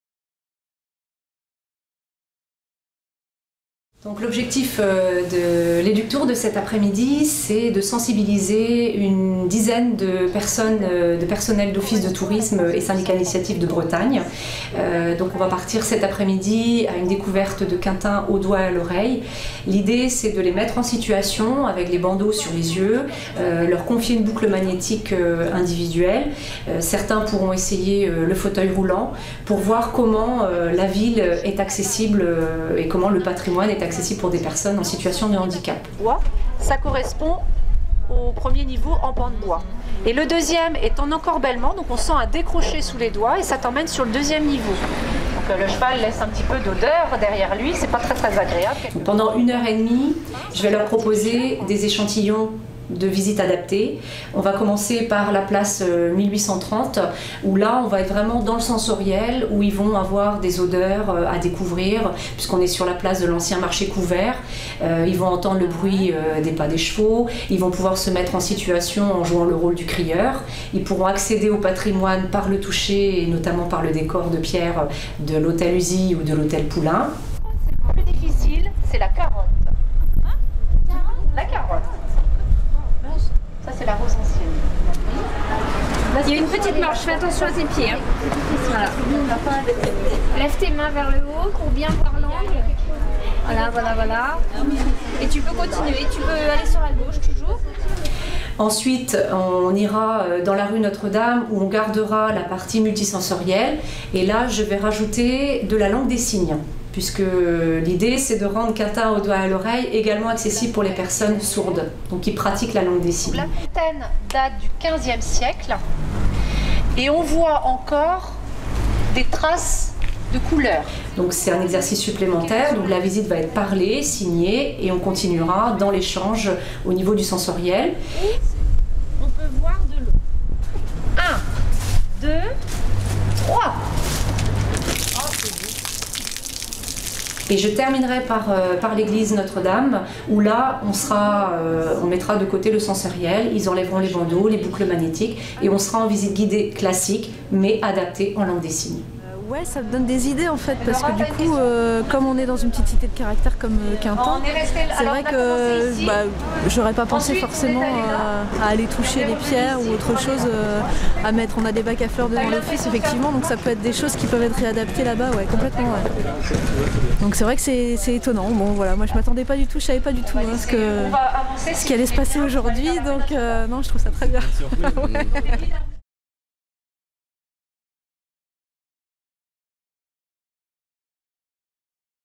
Journée de sensibilisation pour les professionnels du Tourisme - "Eductour"
Les professionnels du tourisme de Bretagne sont venu participer, à Quintin, à 2 journées de sensibilisation aux visites multi-sensorielles que propose Marie-Christine Favennec, responsable d'accueil et guide conférencière à l'Office de Tourisme.